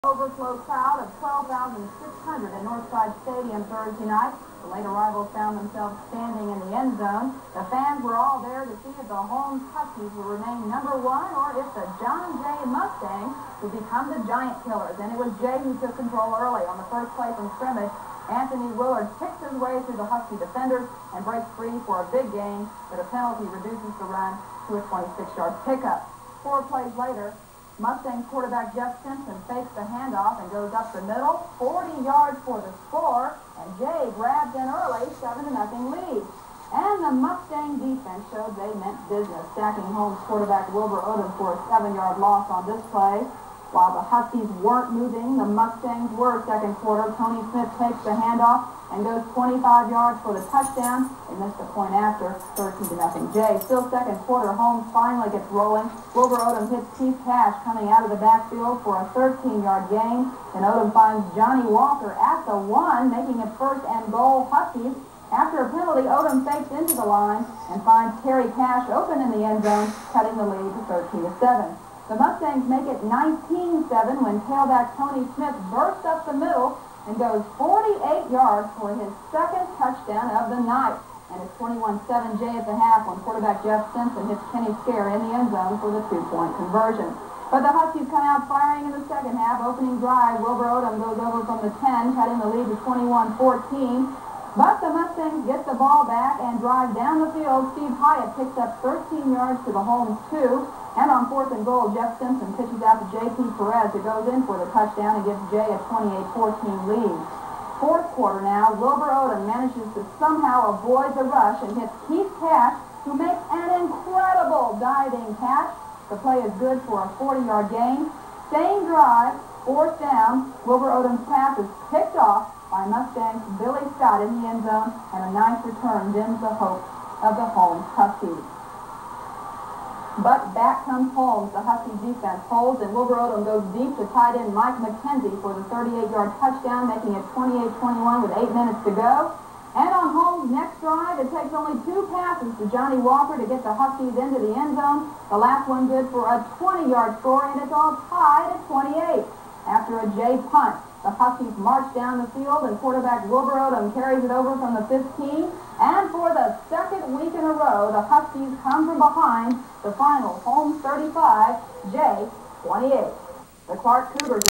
Classic Texas HS Football Game - 1986 Holmes Huskies vs. John Jay Mustangs
Long lost highlights from one of the top high school football games in San Antonio, Texas, history. The game took place on Nov. 6, 1986, and pitted the 9-0 Holmes Huskies against the 8-1 John Jay Mustangs. Nearly 15,000 people were in attendance to watch Holmes erase a 14-point 4th quarter deficit to win 35-28 on a last minute TD run by QB Wilbur Odom. Holmes was ranked #1 in Texas and #2 in the nation by USA Today going into the game.  Holmes team featured future NFL stars Keith and Kerry Cash (identical twins who played tight end and wideout for the University of Texas and then six seasons each for Indianapolis Colts and Kansas City Chiefs), Wilbur Odom (All-American HS quarterback who played for the Michigan Wolverines and Angelo State), Johnny Walker (All-American wideout and outfielder for University of Texas, drafted by Green Bay and Chicago Cubs), Mike McKenzie (tight end who played for the Baylor Bears and was drafted by the Philadelphia Eagles), Trey Martinez-Fischer (member of the Texas House of Representatives), and Sean McKenna (kicker for Air Force Academy and Southwest Texas State), among others.